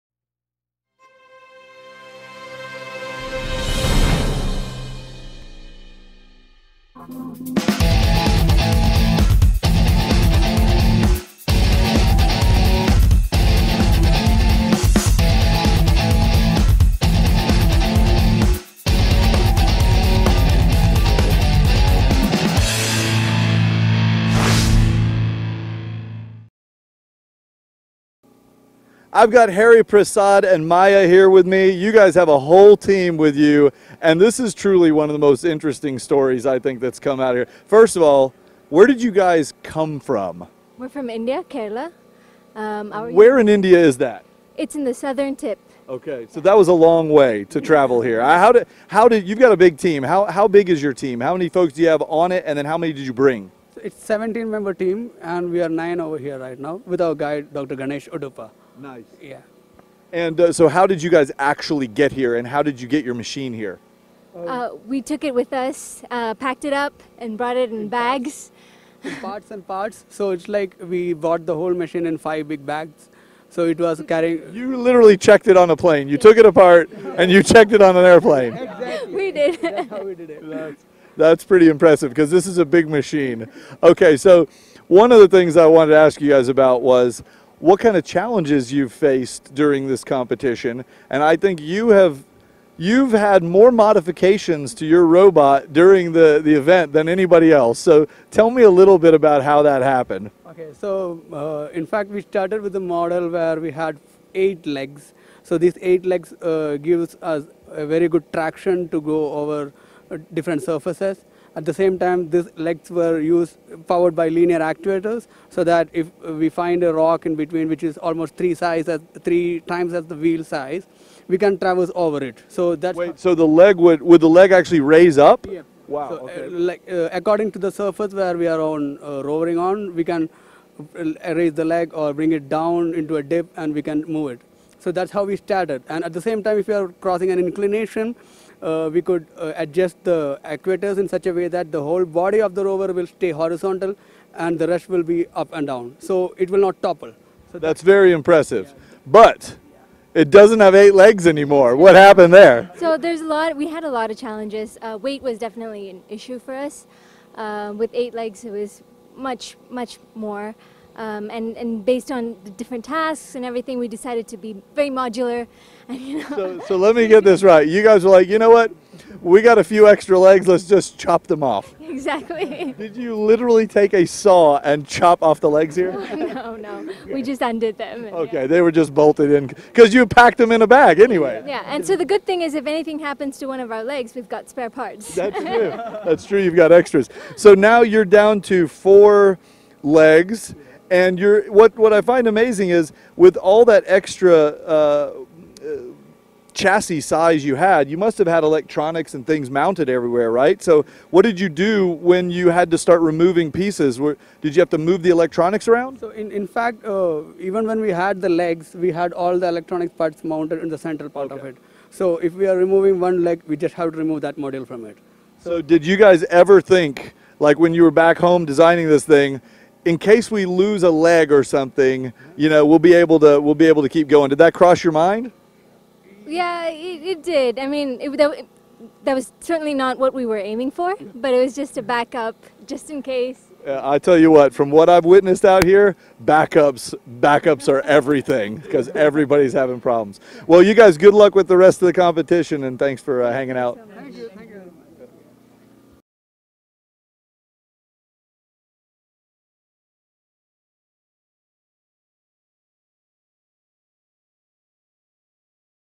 URC 2015 Episode 2: The Rover Boyz Interview
Hariprasad and Maya from Amrita University (team name: The Rover Boyz) chat with GeekBeat's John P about the extraordinary logistics involved with getting to the 2015 University Rover Challenge and being able to adapt their rover design for each competition task.

Protocase was a proud sponsor of the 2015 University Rover Challenge.